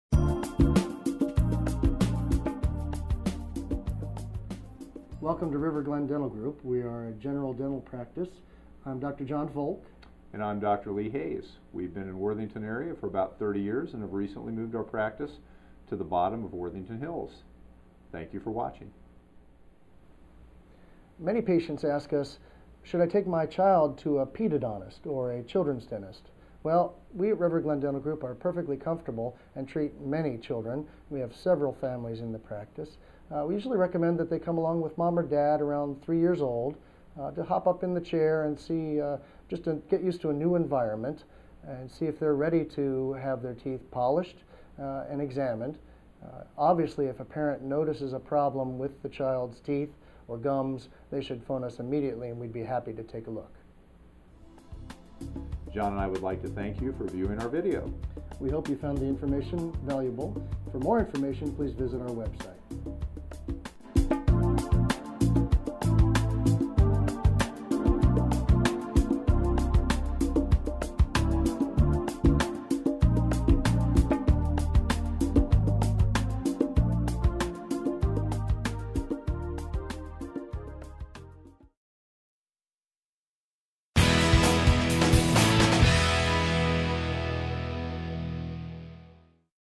Worthington Ohio Dentist - Children and going to the dentist
Worthington OH Dentist - In this episode, Dr. Hays and Dr. Folk discuss issues regarding good oral health. This episode: children and going to the dentist